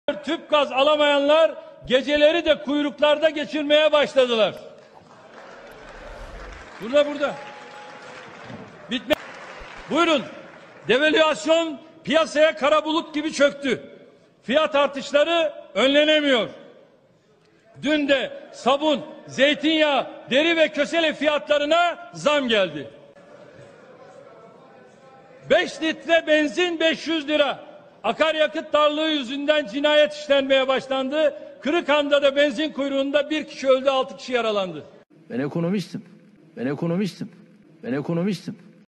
Erdoğan yıllar önce böyle anlatmıştı. Yoksa kınadığını mı yaşıyor? ''Fiyat artışları önlenemiyor.''
Erdoğan yıllar önce zamları böyle anlatmıştı...